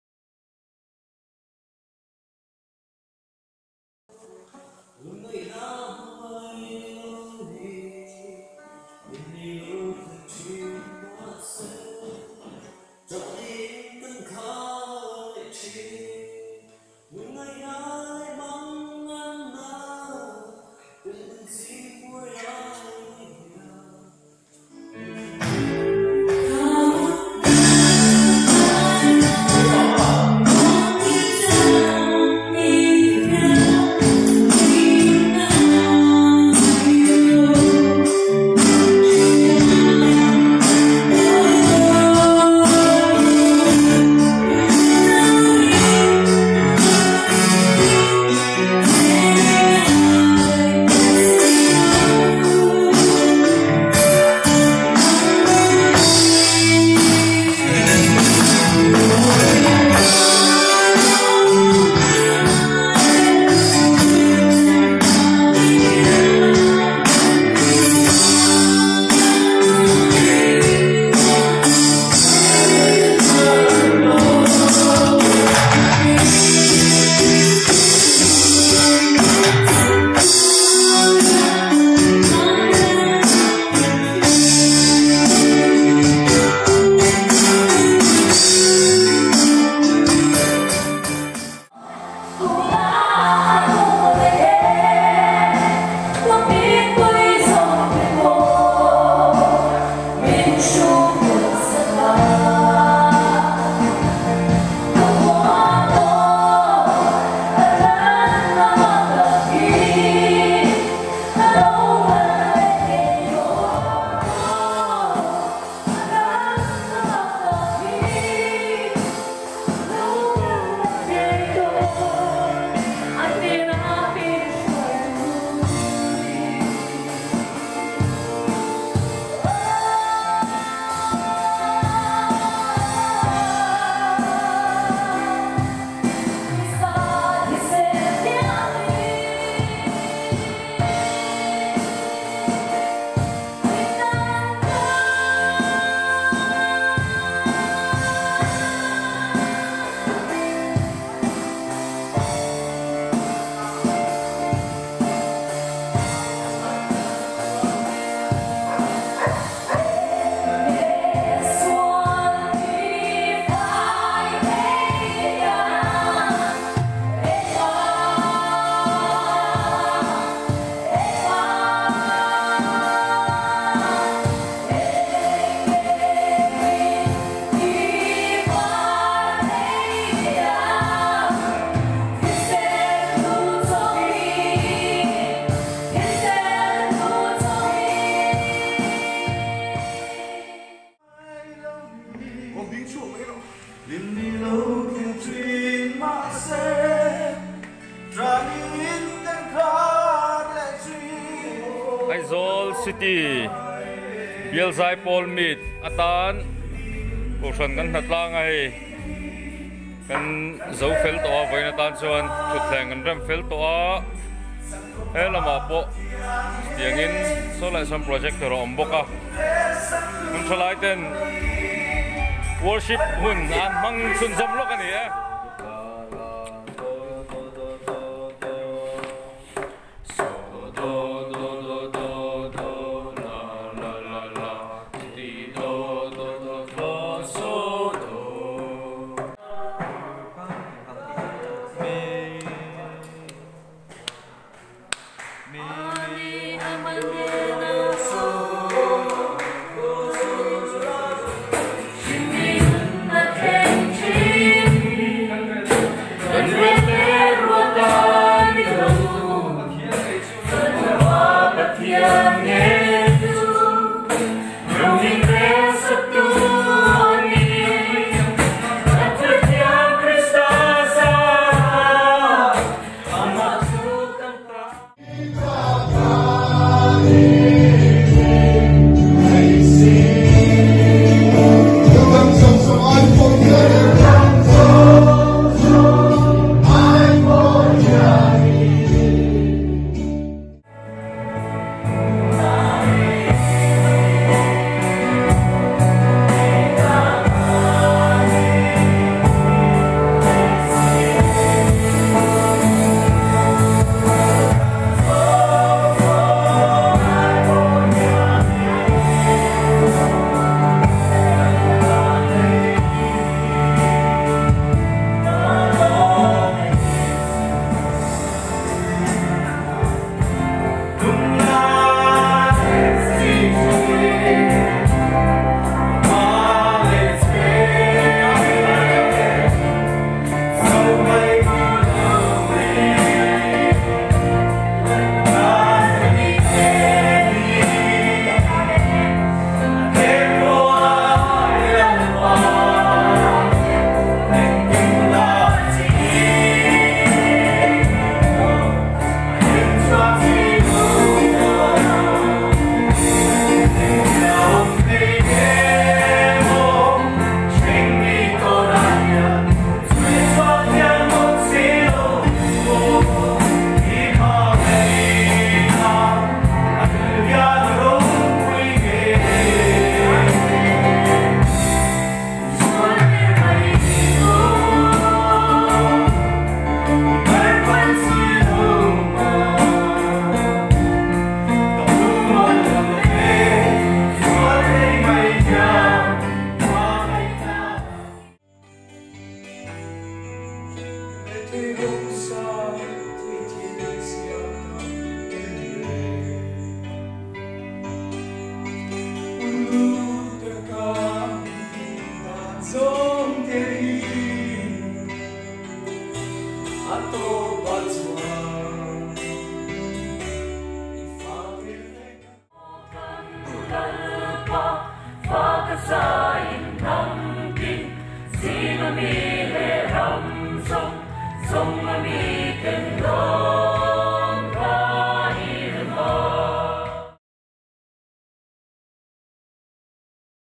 Video hlui lawrkhawm Part 3
Kanaan Unit video hlui - Pathian faka zaina deuh bik remkhawm a ni a, hlazir/enchhinna(rehearsal) video a ni deuh ber awm e.

KRISTA LEH KOHHRAN TAN